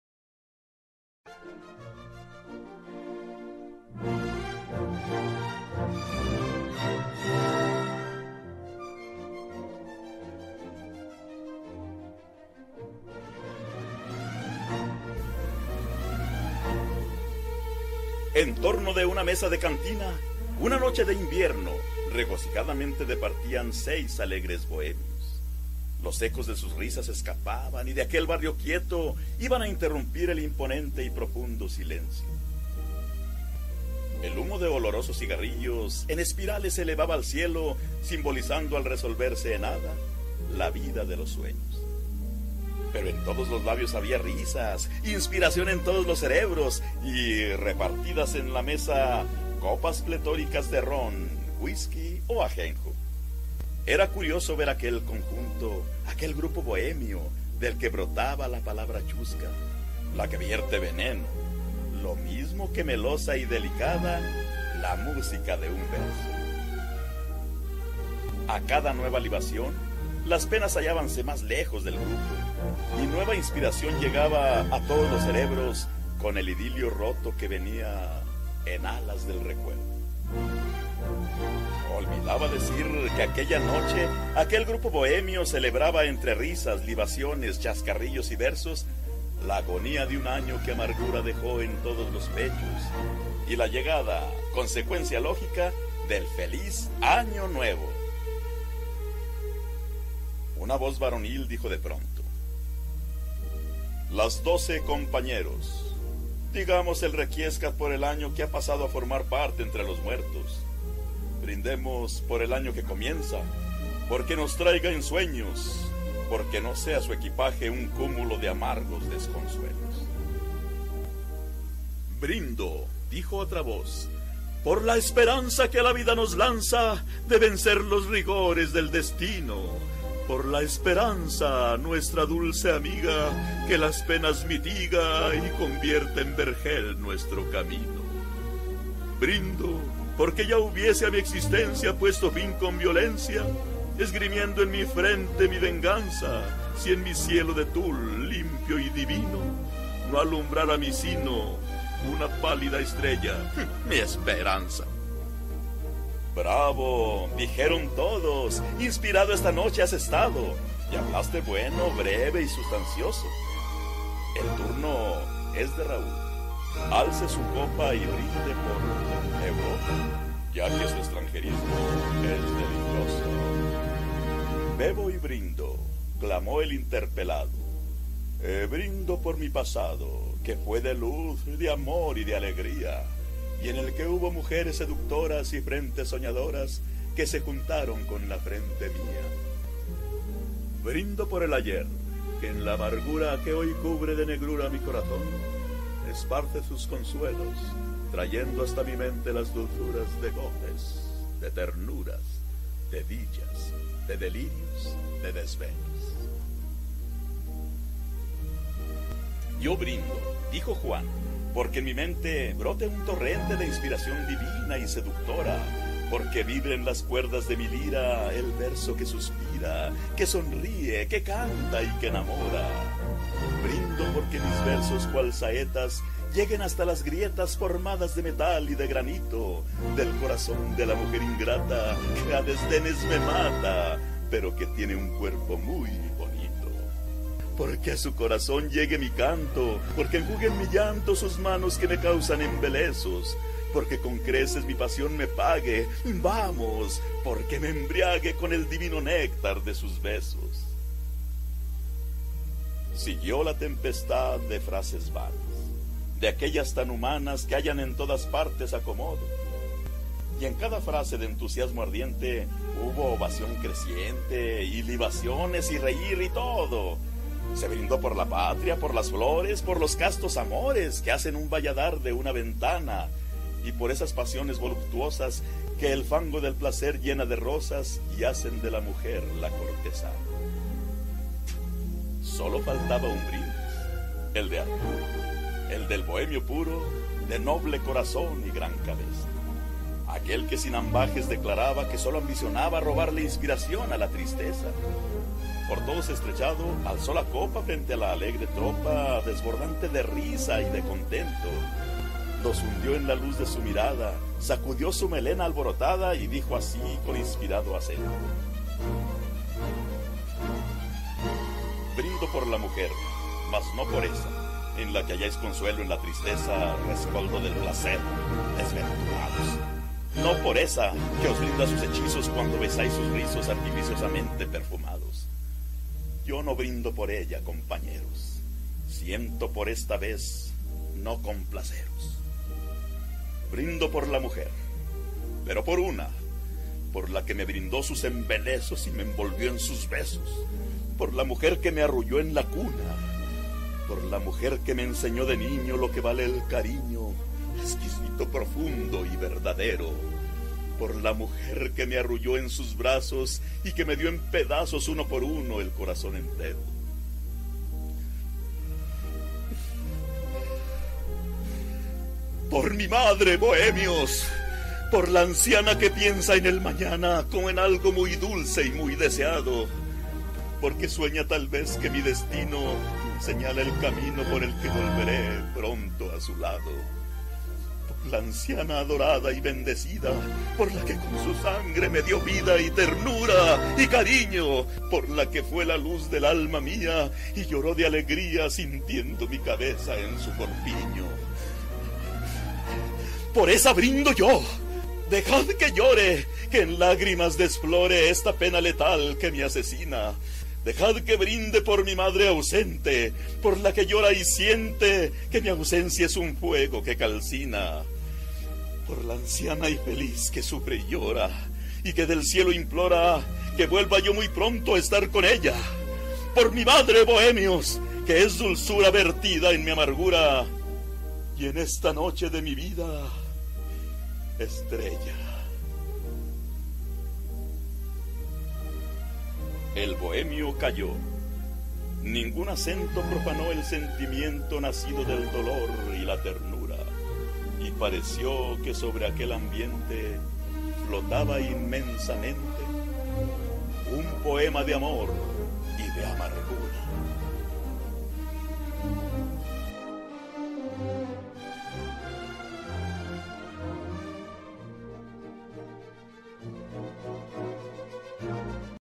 José Luis Salas Romo " EL BRINDIS DEL BOHEMIO"
El brindis del Bohemio Obra maestra del Poeta y periodista Potosino  GUILLERMO AGUIRRE Y FIERRO